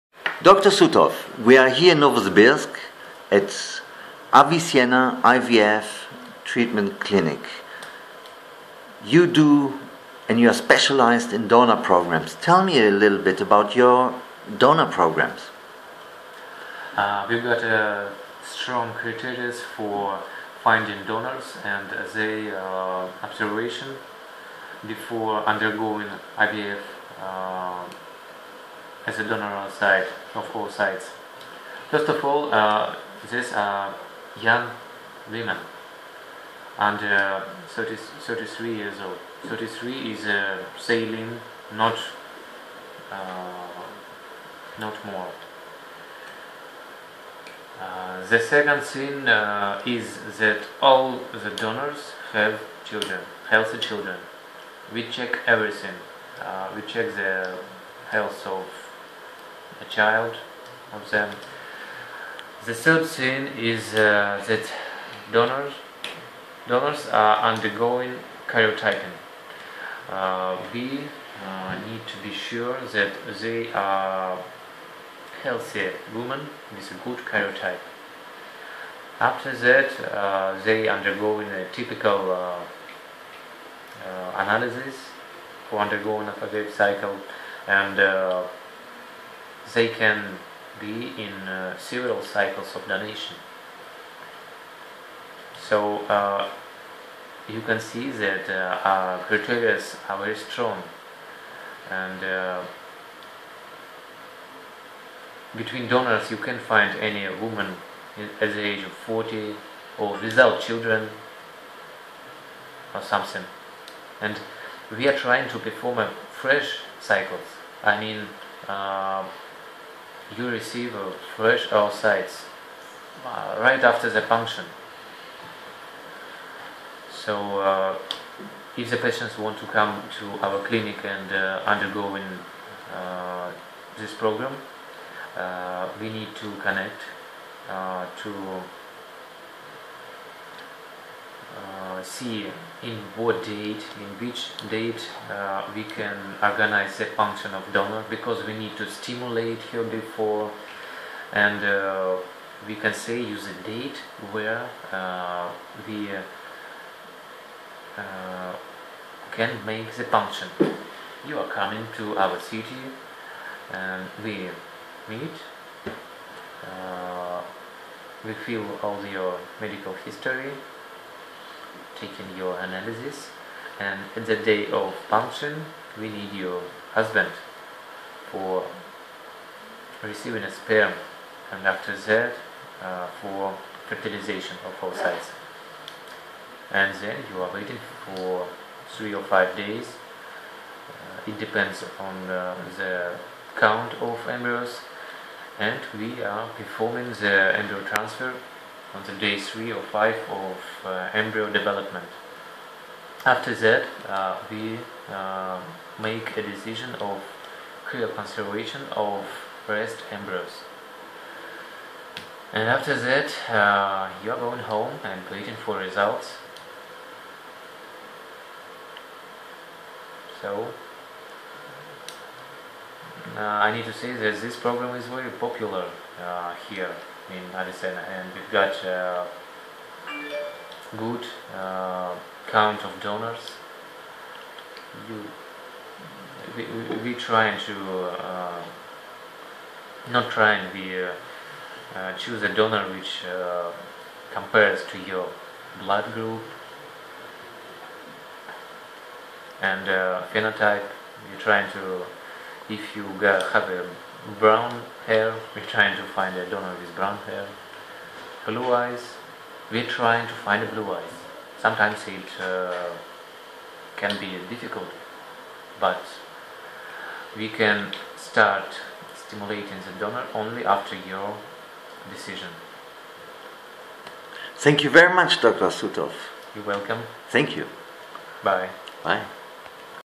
IVF Clinic Avicenna Dr. Zotov - Novosibirsk
Dr. Zotov from the Avicenna IVF Clinic in Novosibirsk explains the donor program and procedure in the clinic.

Dr. Zotov is specialized in Gynecology and Reproduction medicine in Novosibirsk - Russia. The leading institution in IVF Treatment.